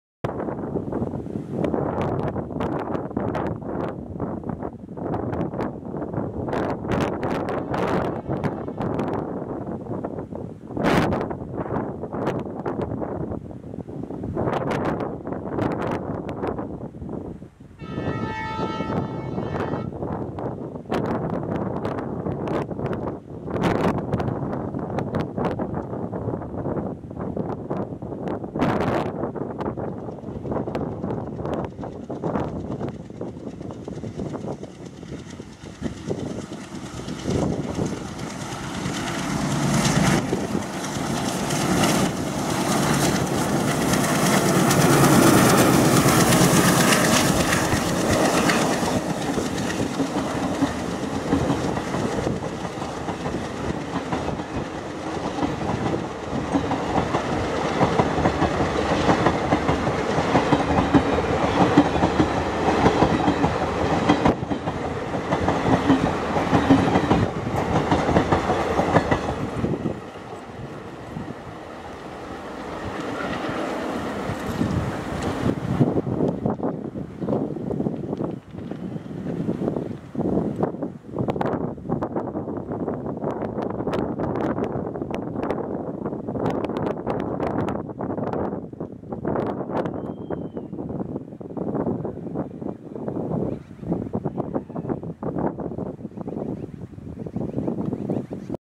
[HD]: GY WDM 3D accelerates past with Chennai Egmore - Dadar Exp
Alco with twin-toned lights chugging like an old WDM 2
Train No: 12164
Train Name: Chennai Egmore (MS) - Dadar (DR) Superfast Express
Loco: GY[GOOTY] WDM 3D #11439
Location: Yermaras (5kms from Raichur)
Camera: Canon Power Shot SX 500IS